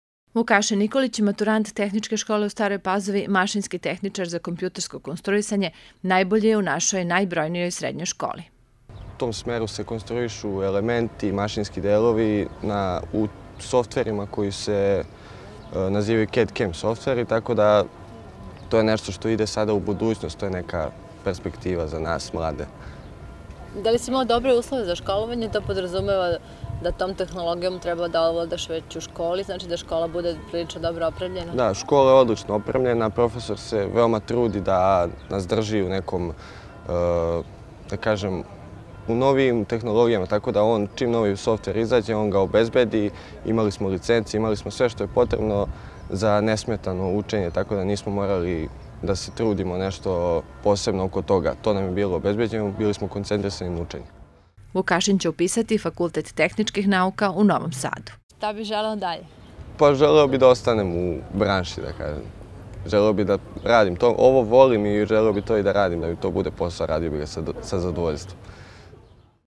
Vukašin Nikolić
Kraj školske godine je vreme kada se među maturantima biraju đaci generacije. U Tehničkoj školi u Staroj Pazovi najbolji među maturantima je Vukašin Nikolić.